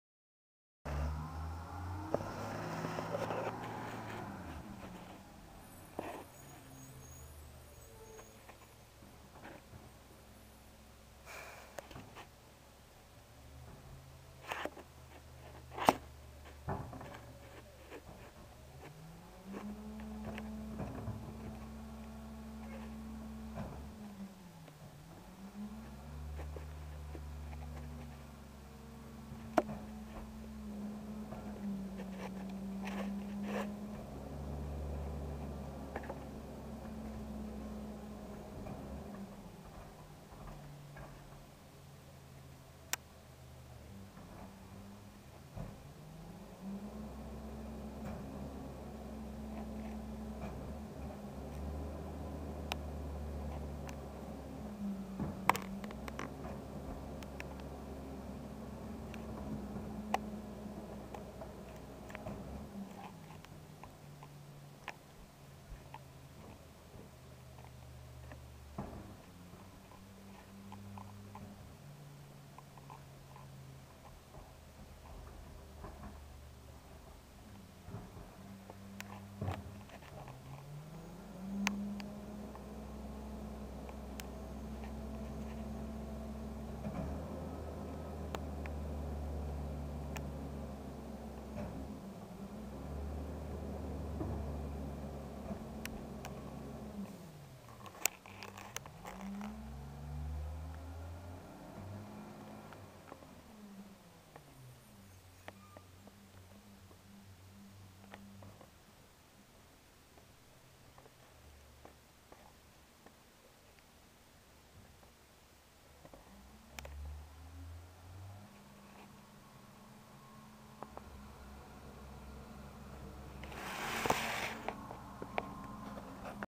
TSI Truck #388 Collecting Yard Waste
Here is truck 388 collecting Yard waste last Tuesday. Sorry for the shaky camera and lower quality, I was not ready for the truck and did not have time to set up the tri-pod.
SS11 Episode IV (1/2)